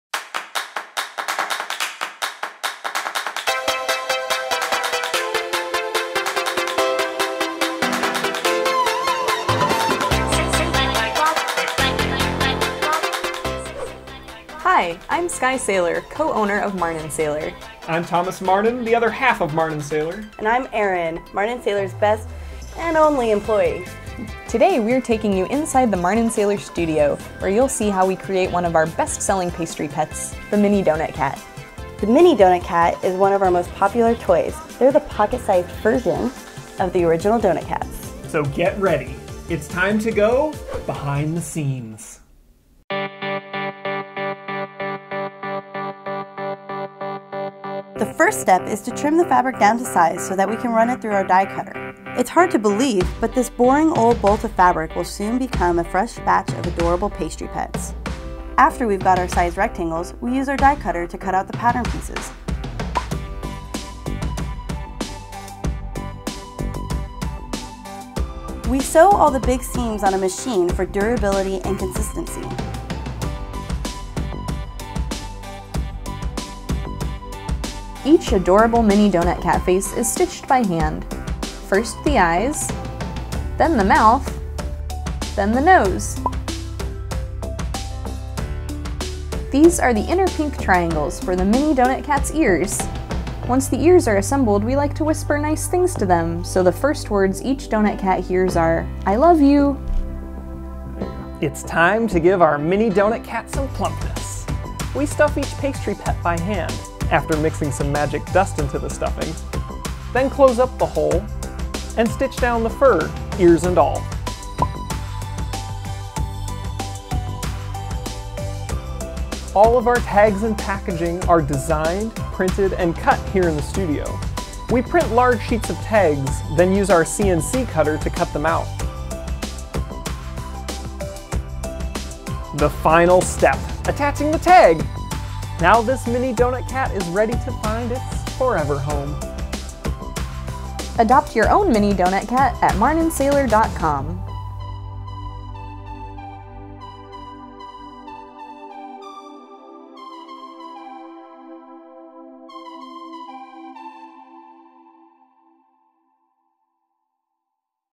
Behind the Seams: Mini Donut Cats
Step inside the MarninSaylor studio and see how we create one of our best selling toys, the Mini Donut Cat. From bolt of fabric to completed Pastry Pet, watch as we take you behind the seams!

Directed, filmed, and edited by James Andrews
Written by Skye Saylor
Starring Erin Weathers, Skye Saylor, and Thomas Marnin
Thanks to Andy Dopieralski and Red Element Studios

Adopt your own at MarninSaylor.com